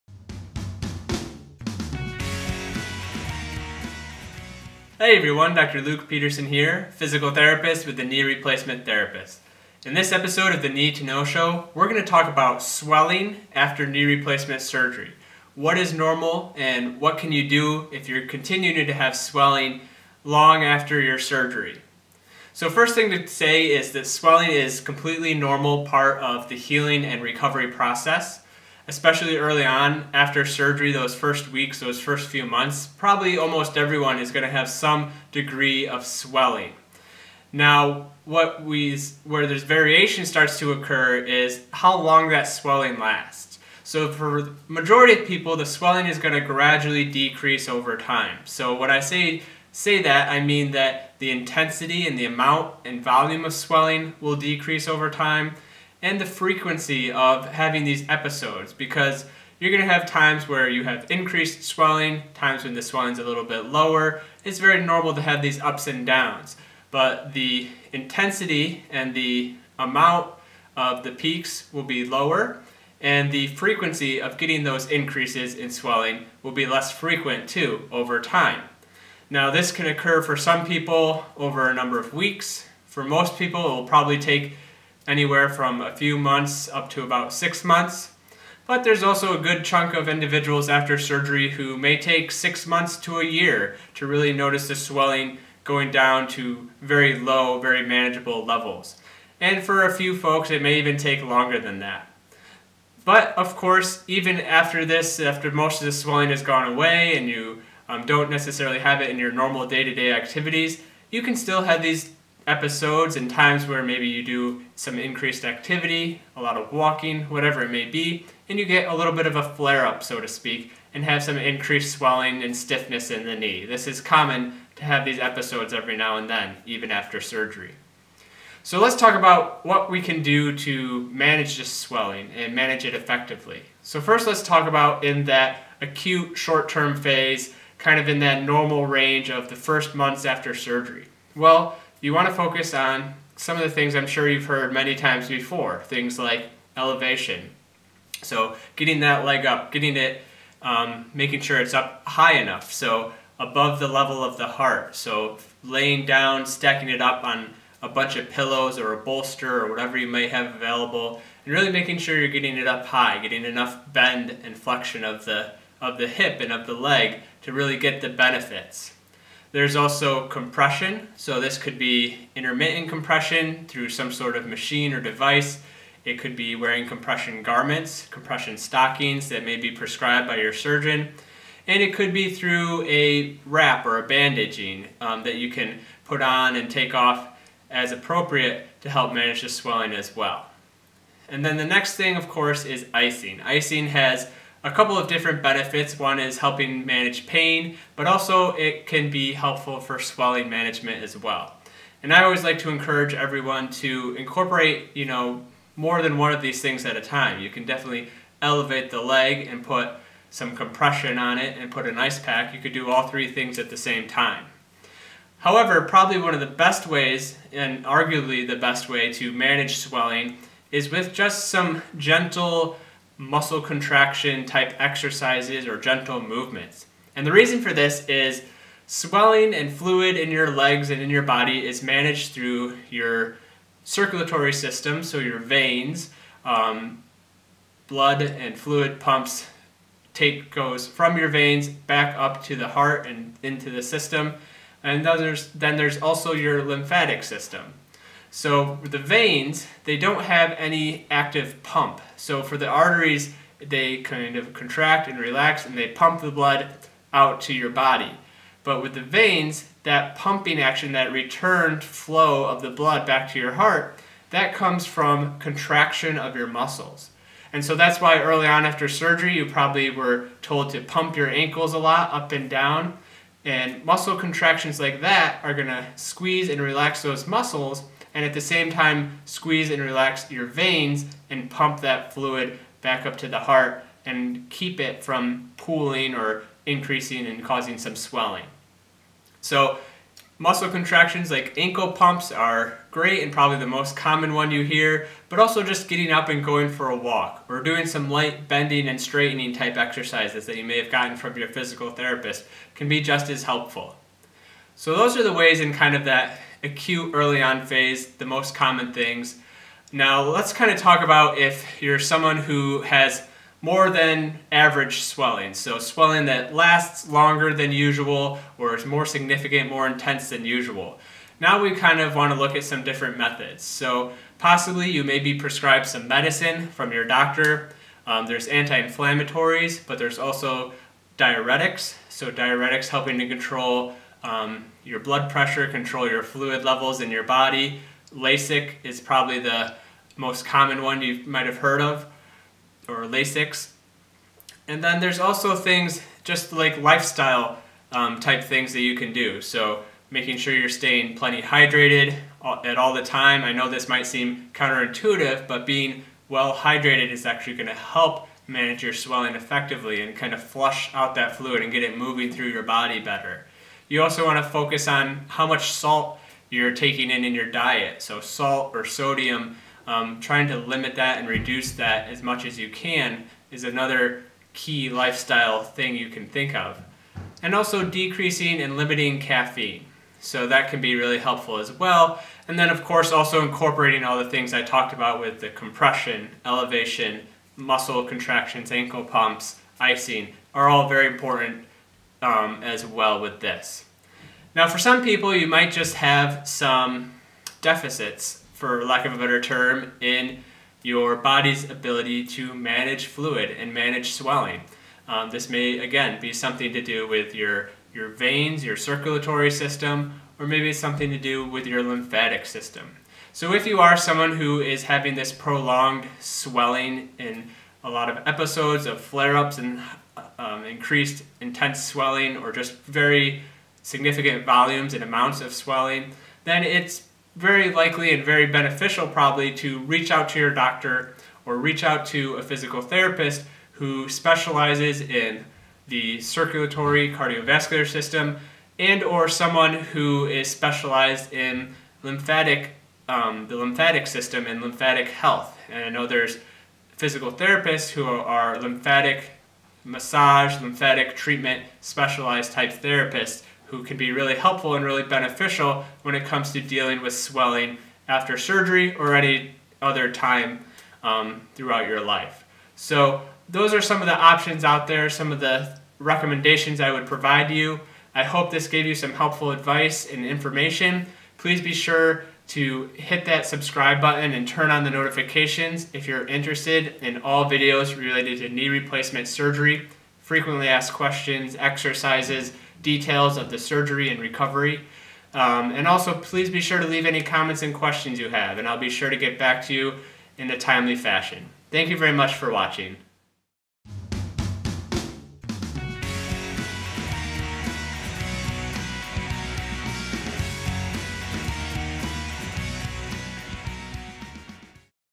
Why is My Knee Still Swollen? - Knee Replacement
In this episode physical therapist Dr. Luke discusses ways to manage swelling both early on after surgery and long-term months and years after surgery.

Knee to Know Ep. 203